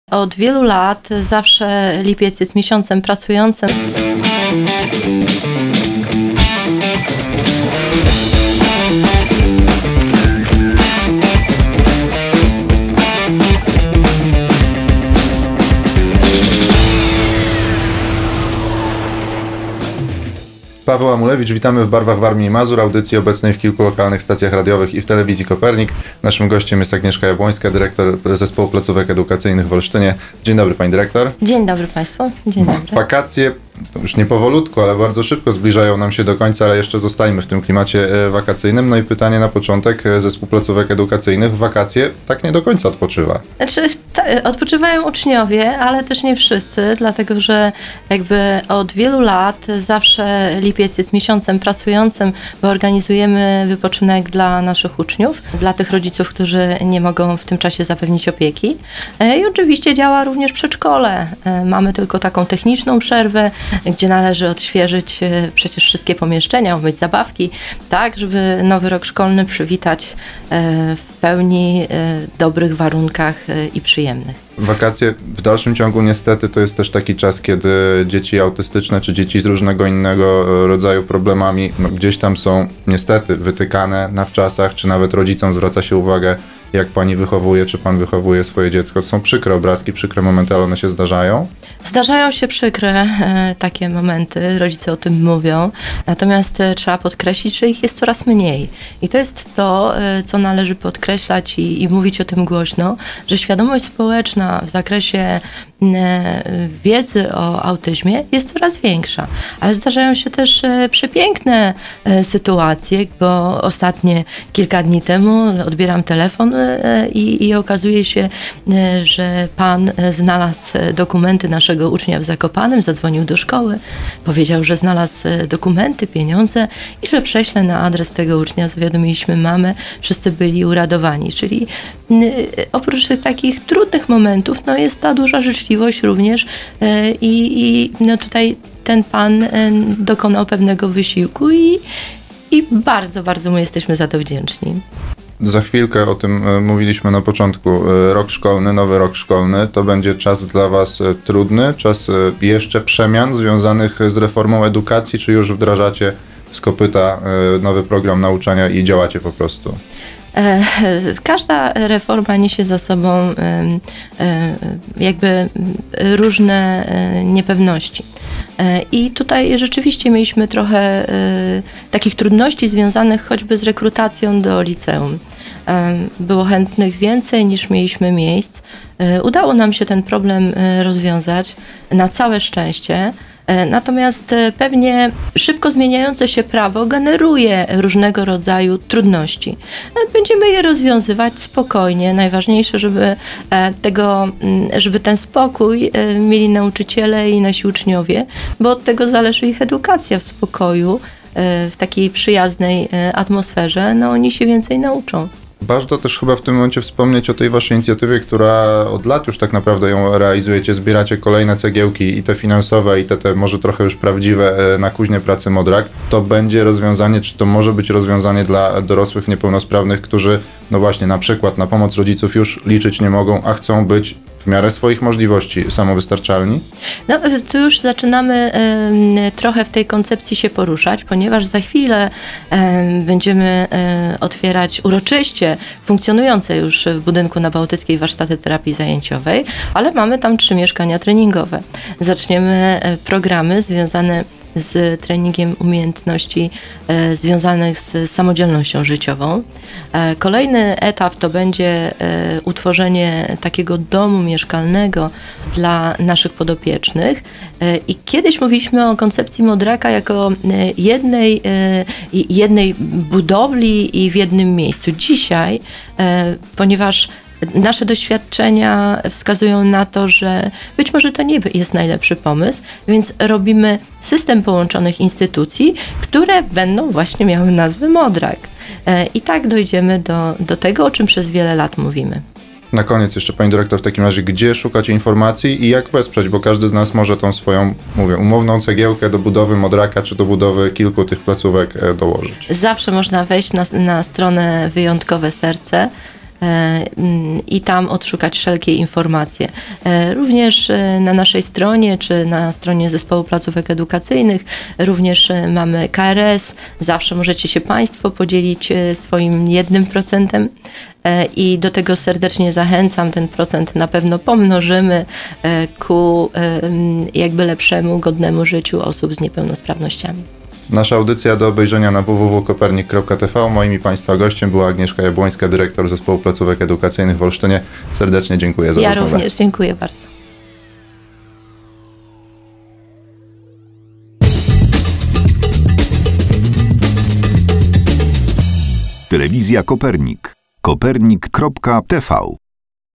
WAKACJE I PO WAKACJACH
Uczniowie Zespołu Placówek Edukacyjnych w Olsztynie wracają w progi swojej ulubionej szkoły. Jak spędzali wakacje i jaką szkołę zobaczą w nowym roku szkolnym?